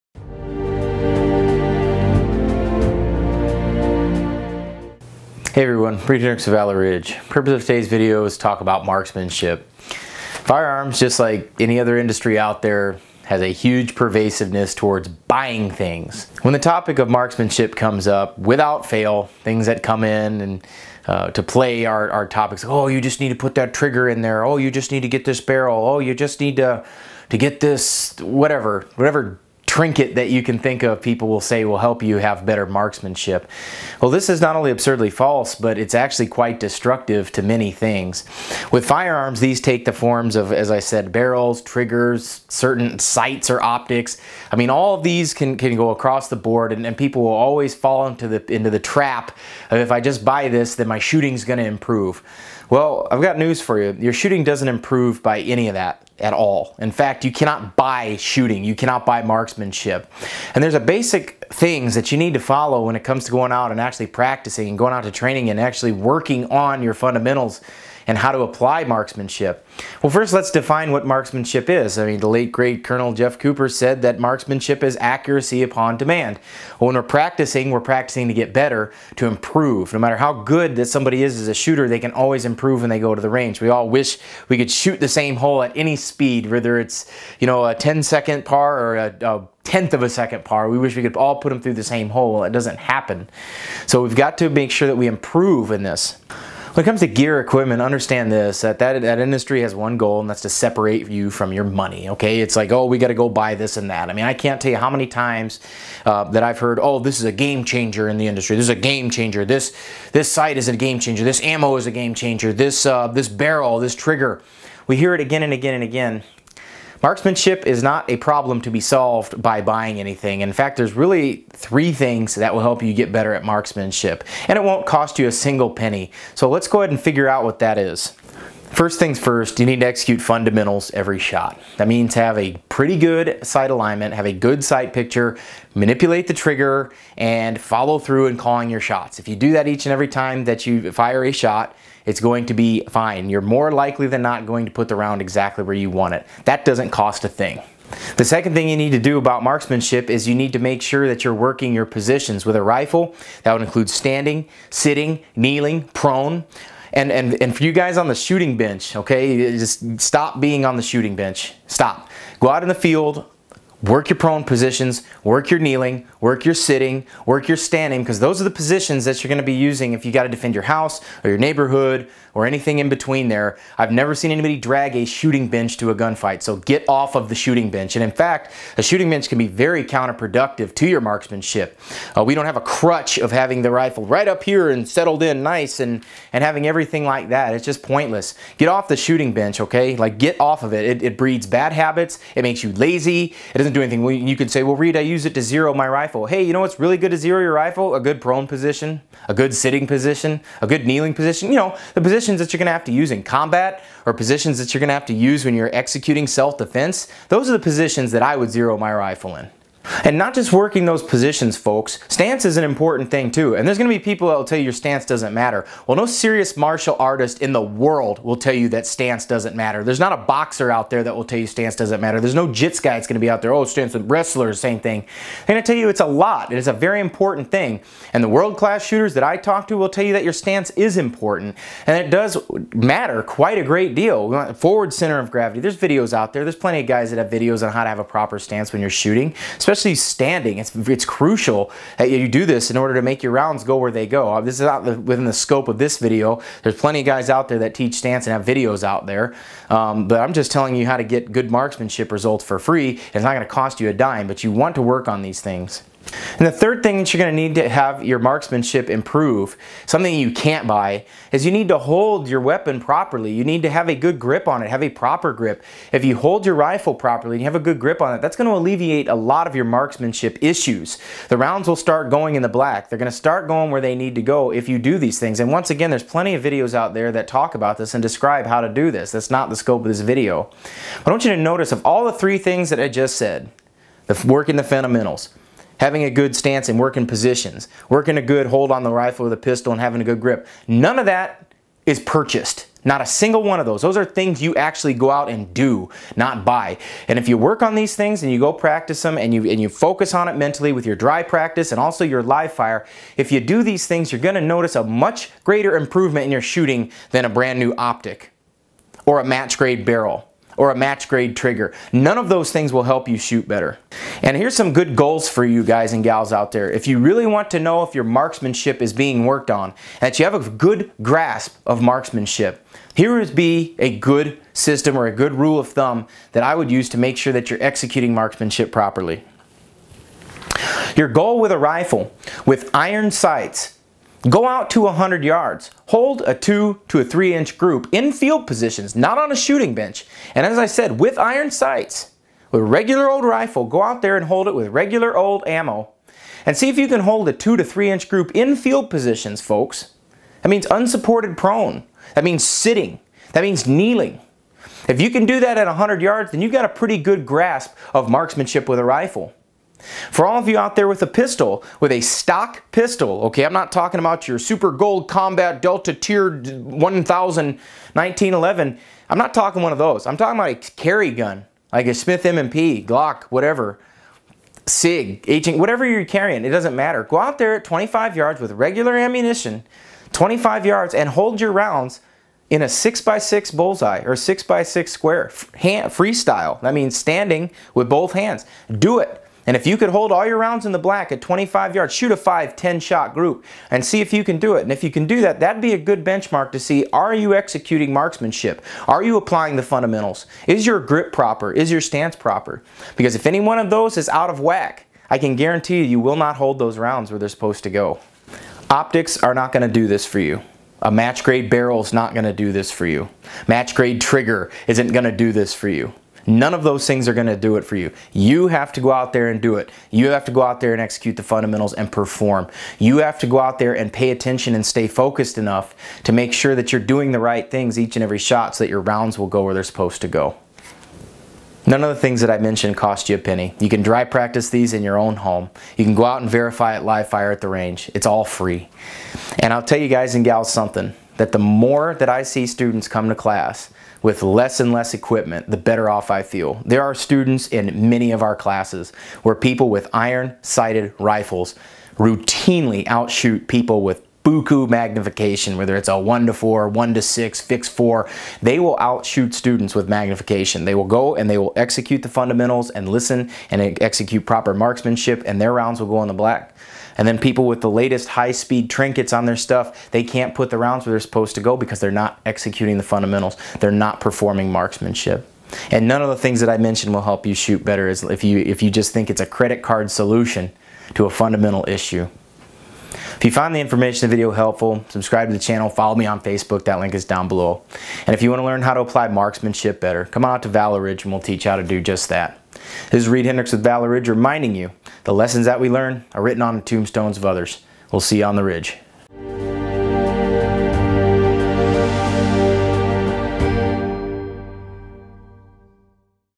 You Can Not Buy Marksmanship
tag=valrid01-20

Marksmanship is something you do, not buy. Many people have tried to purchase an "edge" or secret piece of equipment that will somehow enable them to hit accurately. But what is missing is a grasp of fundamentals, stance, grip, and positions. YOU must do the work, not your credit card. YOU must perform and execute fundamentals each and every shot, and YOU must be focused and disciplined with your mind and body.